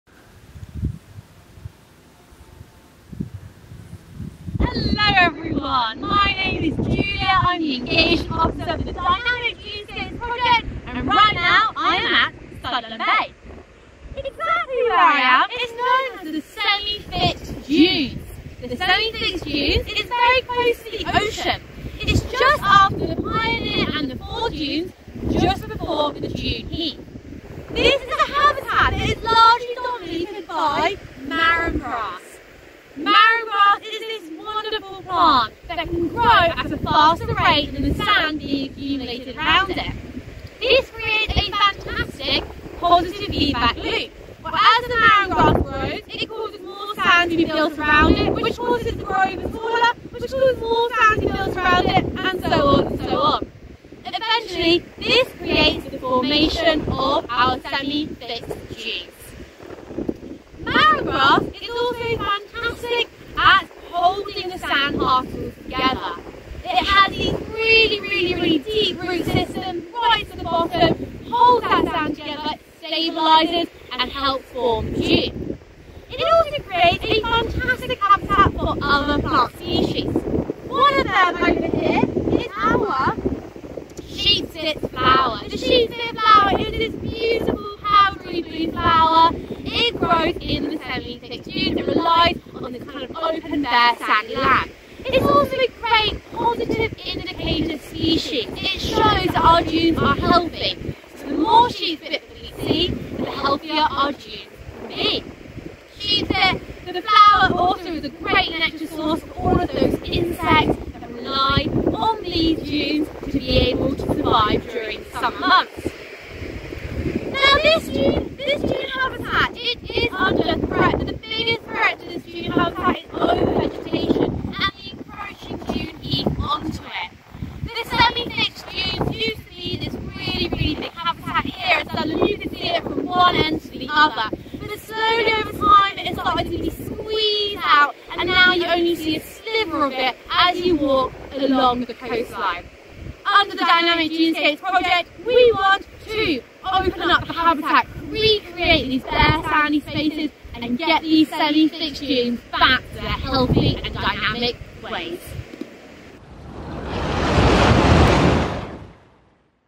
Habitat Type: Semi Fixed Dunes at Studland Bay
In this video, Julia Galbenu from Studland Bay National Trust guides you through the formation of semi-fixed dunes, introduces you to the kinds of life that are adapted to live in this type of habitat, and explores the work that we're doing to protect and extend this habitat type.